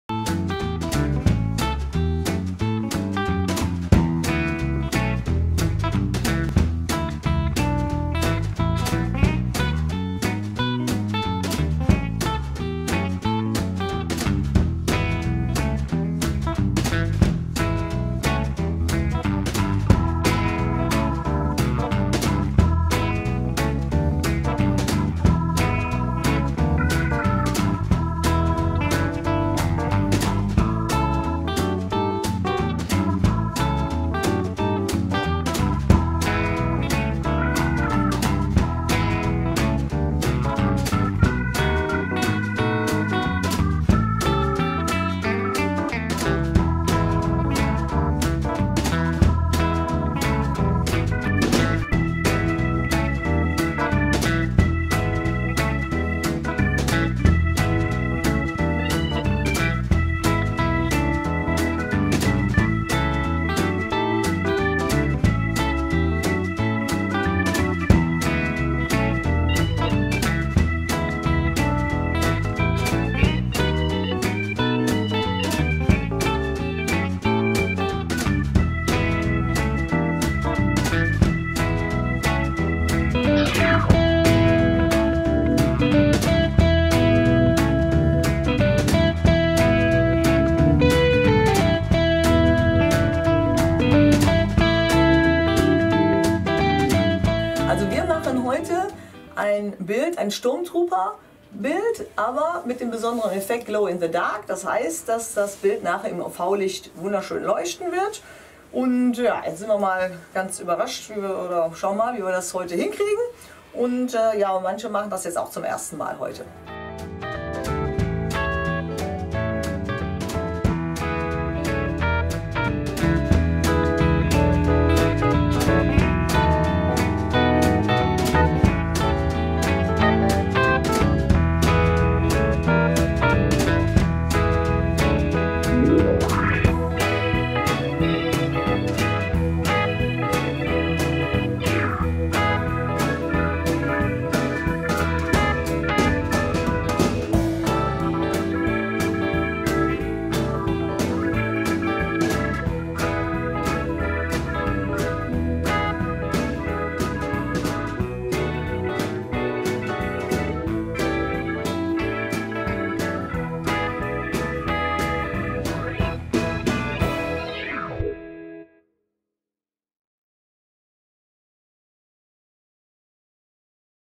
Ferienwoche 5. und 6. Graffiti 2021
Seit 2013 bieten wir Graffiti-Wochen an und weil es so viel Spaß macht, dann auch gleich zwei Wochen. Ca. 200 Dosen wurden versprüht und ergeben jetzt wunderschöne Bilder an unserer Fußballmauer und neben dem Eingangstor. Dazu kommen sehr viele Bilder, die jetzt einige Wände verschönern.
Das ist der zweite Film von Filmteam-Florian und gefördert von der Telekom-Stiftung.
Wir bedanken uns auch bei der Firma Molotow-Feuerstein GmbH, dass sie uns so schnell noch Dosen nachgeliefert hatte, nachdem manche Farben doch knapp wurden.